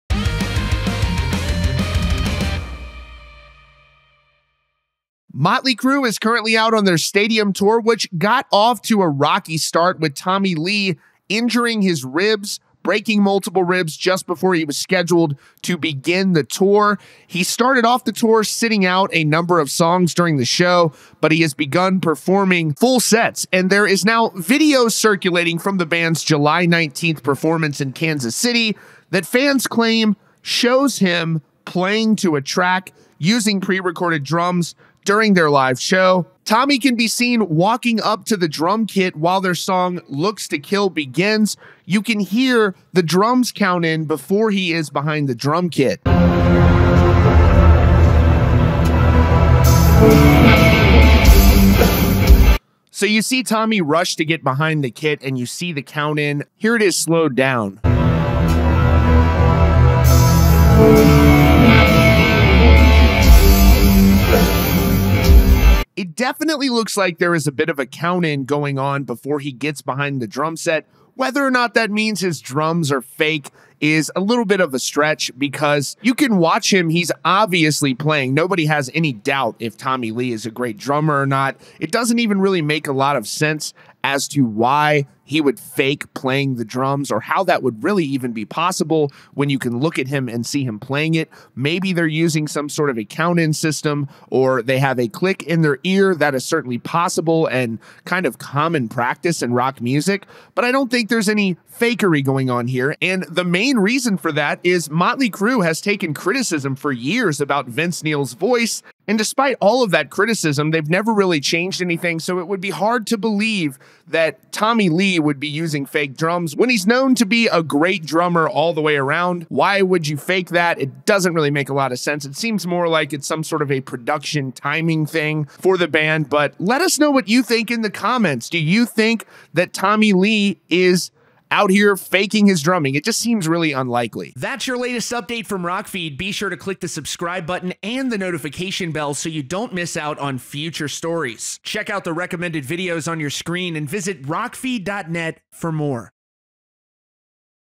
Motley Crue's Tommy Lee Caught Using Pre Recorded Drums?
Motley Crue drummer Tommy Lee has been accused of using pre-recorded drums after a clip circulating shows the drums counting in while he isn't behind the kit. Motley Crue is currently out on The Stadium Tour.